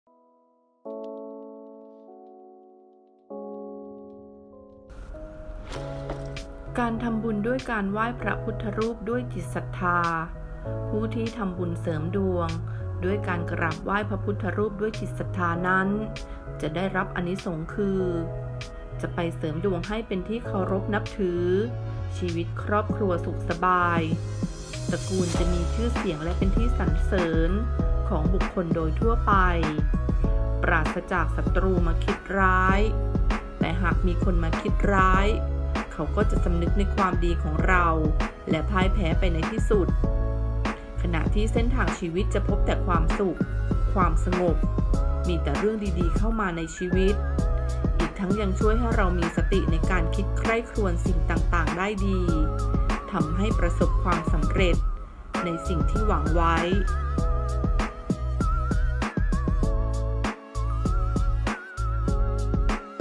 ไหว้พระพุทธรูปด้วยจิตศรัทธา ช่วยเสริมดวงชีวิตสบาย
ผู้ที่ทำบุญเสริมดวงด้วยการกราบไหว้พระพุทธรูปด้วยจิตศรัทธานั้น จะได้รับอานิสงส์คือ จะไปเสริมดวงให้เป็นที่เคารพนับถือ ชีวิตครอบครัวสุขสบาย ตระกูลจะมีชื่อเสียงและเป็นที่สรรเสริญของผู้คนโดยทั่วไป ปราศจากศัตรูมาคิดร้าย แต่หากมีคนมาคิดร้ายเขาจะสำนึกในความดีของเราและพ่ายแพ้ไปในที่สุด  ขณะที่เส้นทางชีวิตจะพบแต่ความสุข ความสงบ มีแต่เรื่องที่ดีเข้ามาในชีวิต อีกทั้งยังช่วยทำให้เรามีสติในการคิดใคร่ครวญสิ่งต่างๆได้ดี ทำให้ประสบความสำเร็จในสิ่งที่หวังไว้